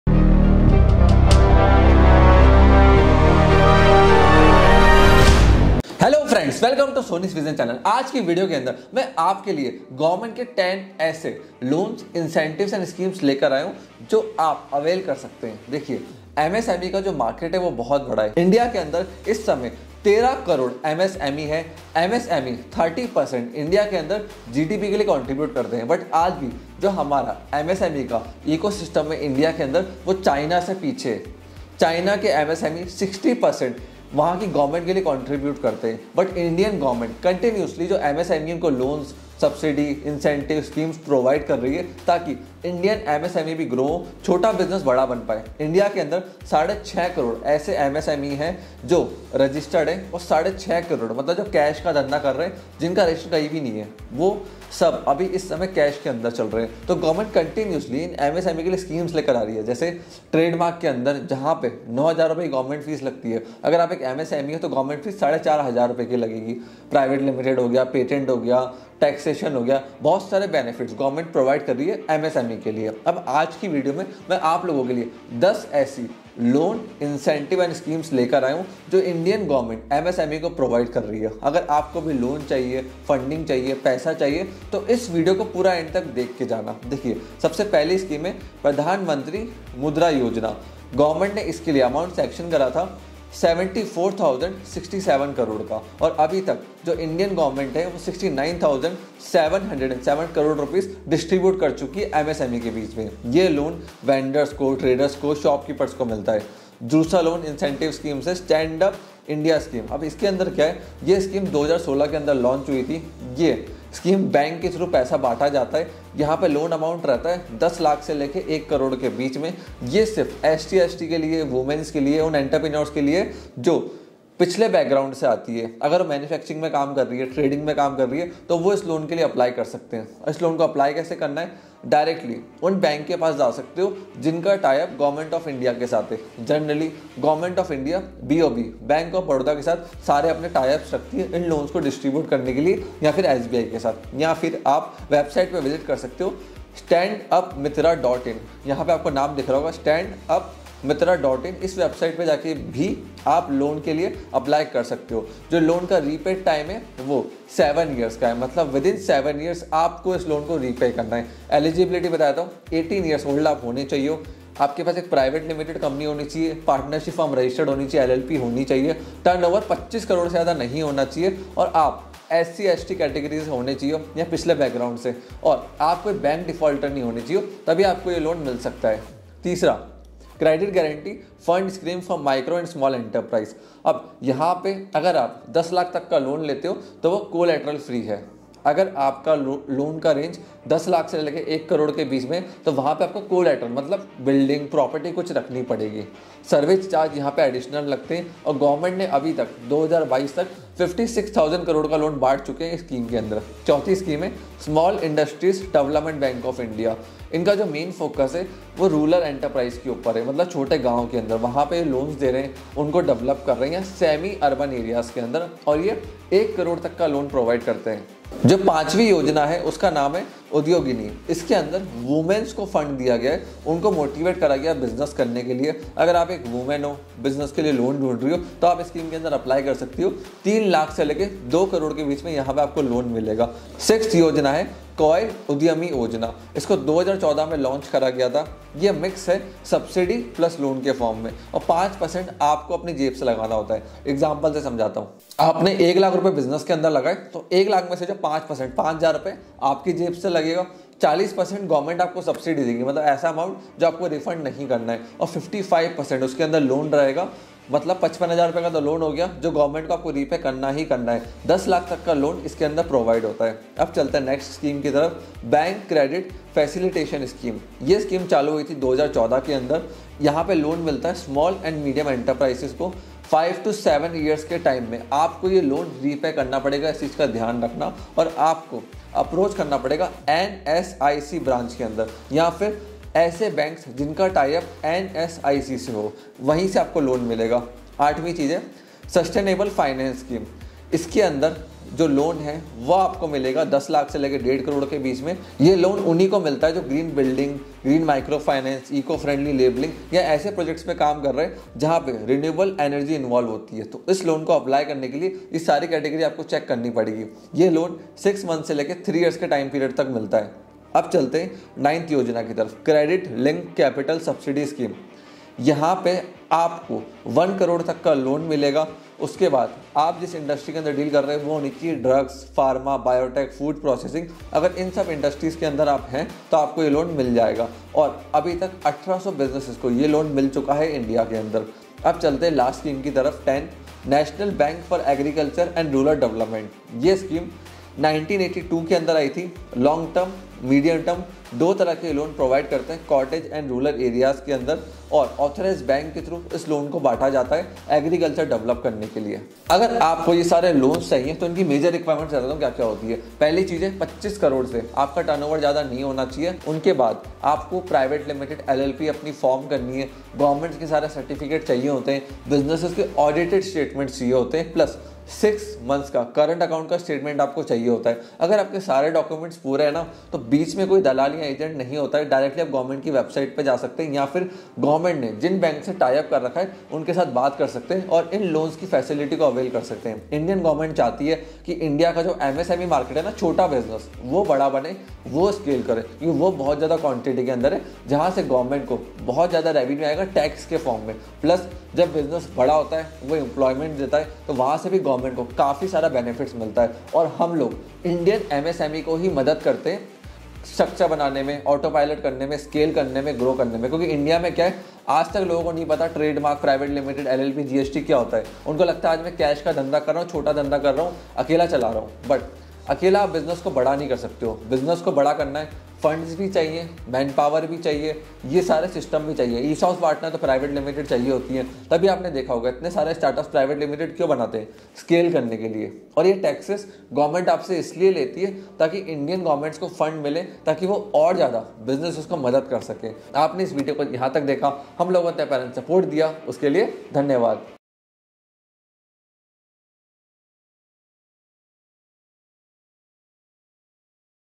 MSME Loans for New Business | 10 Government Loan Scheme for MSME
Top 10 MSME and Startup Business Loans by Government of India :

The Micro, Small, & Medium Enterprises (MSMEs) contribute to 29% of India’s GDP, and are responsible for almost half of the nation’s exports. On the other hand, startups have been mushrooming across the country and are a massive contributor to taking India higher on the global pedestal. India has become the third-largest startup ecosystem in the world after the US and China.

Did you know that India has more than 7.9 million micro, small and medium enterprises (MSMEs) as of 27 March 2022, according to the Ministry of Micro, Small & Medium Enterprises?

Pradhan Mantri Mudra Yojana
Stand Up India Scheme
Credit Guarantee Fund Scheme for Micro and Small Enterprises
Small Industries Development Bank of India
Udyogini
Coir Udyami Yojana
Bank Credit Facilitation Scheme
Sustainable Finance Scheme
Credit-Linked Capital Subsidy Scheme (CLCSS)
National Bank for Agriculture and Rural Development
How To Register For Government Loan Schemes?

Applying for the MSME and startup business loans provided by the Government of India is very similar to other loans. There are two ways to go about it:

You can visit the lender’s website and fill in the online application form. You will be required to submit all necessary documents either online or offline, depending on the lender’s rules.

You can visit the lender’s offline/physical branch and fill out their application form and the required documents.
Common Eligibility Criteria to Register for a Startup Business Loan by The Government of India

The company should not be older than five years in the case of existing enterprises

The total turnover of the business should not be more than INR 25 crores

Only Limited Liability Partnerships or Private Limited companies are eligible for these loans

Eligible applicants should also have approval for DIPP (Department of Industrial Policy and Promotion)

A patron guarantee from the Indian Patent and Trademark Office is a must to qualify for Government startup loans